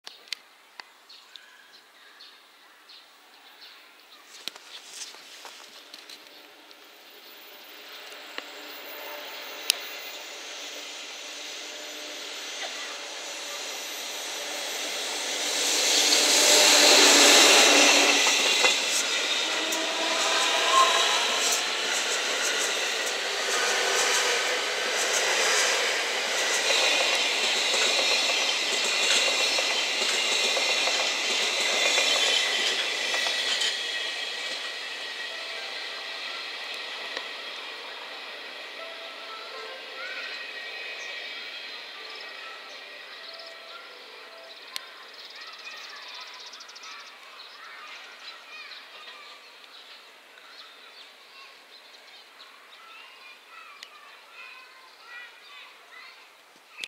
カープ電車と重なった 山陽本線 上り 工事臨時列車 工9380レ EF65-1135号機 機関車 下関 工臨 ロンチキ レール貨物 12両編成 JR西日本 中野東 2020.3.30 01701
山陽本線 上り 工事臨時列車 工9380レ EF65-1135号機 機関車 下関 工臨 ロンチキ レール貨物 12両編成 JR西日本 中野東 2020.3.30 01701